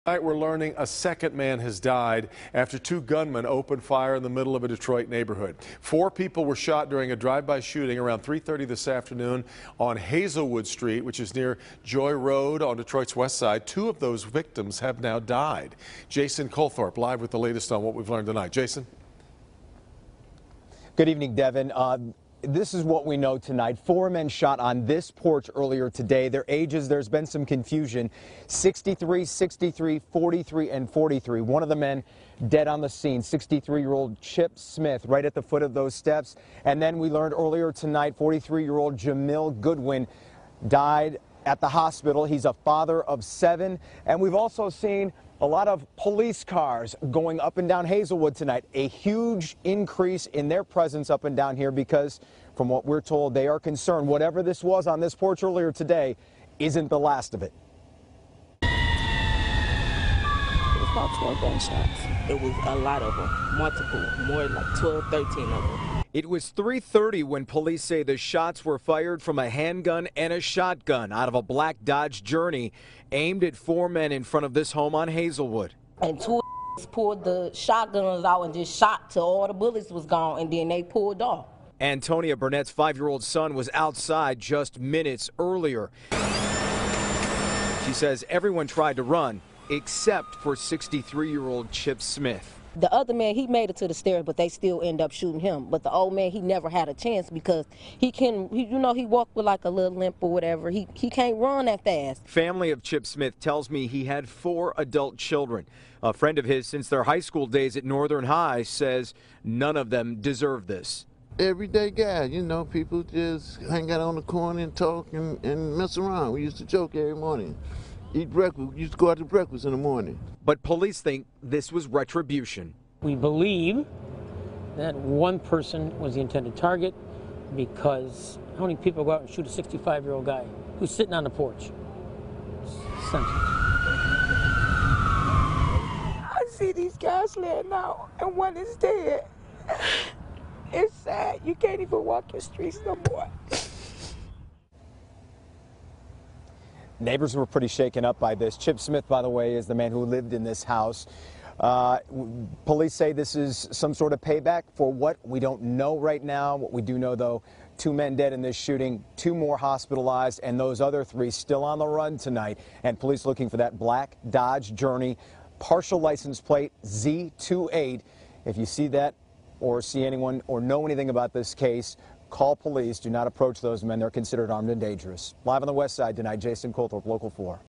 Four people shot, two killed on Hazelwood St. on Detroit’s west side
Detroit police are looking for two gunmen in a black minivan who opened fire on four people who were sitting on a porch on the city's west side, killing one of them.